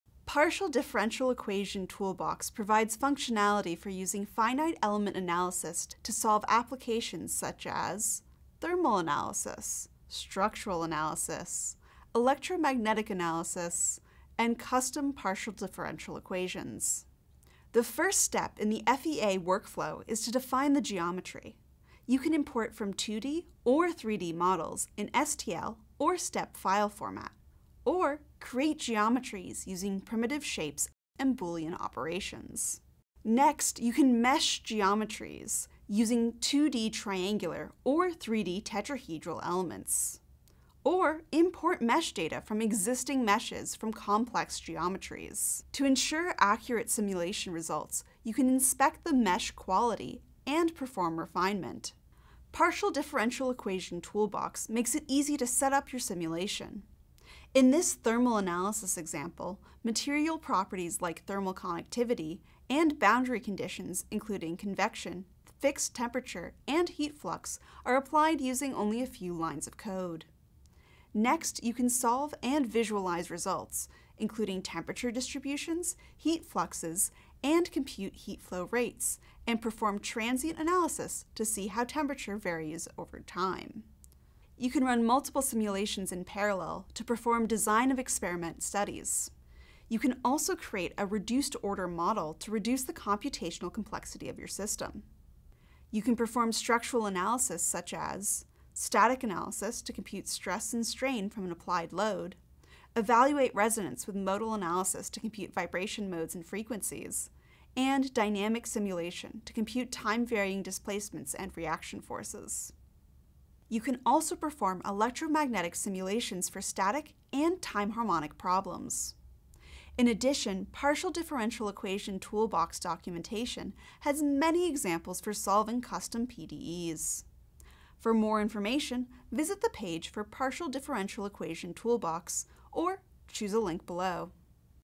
What Is Partial Differential Equation Toolbox?
Partial Differential Equation Toolbox provides functions for solving heat transfer, structural mechanics, electromagnetics, and general partial differential equations (PDEs) using finite element analysis.

You can model conduction-dominant heat transfer problems with convection and radiation occurring at boundaries and account for surface-to-surface radiation. You can calculate temperature distributions, heat fluxes, and heat flow rates through surfaces. You can perform linear static analysis to compute deformation, stress, and strain. For modeling structural dynamics and vibration, the toolbox provides modal and direct time integration solvers and frequency response analysis. You can analyze a component’s structural characteristics by performing modal analysis to find natural frequencies and mode shapes. You can perform electrostatic, magnetostatic, DC conduction, and harmonic analyses and solve a variety of standard problems using templated second-order PDEs.

Partial Differential Equation Toolbox lets you import 2D and 3D geometries from STL or STEP files, reconstruct geometries from mesh data, or build geometries from primitive shapes. You can automatically generate meshes with triangular and tetrahedral elements. You can solve PDEs by using the finite element method, and postprocess results to explore and analyze them.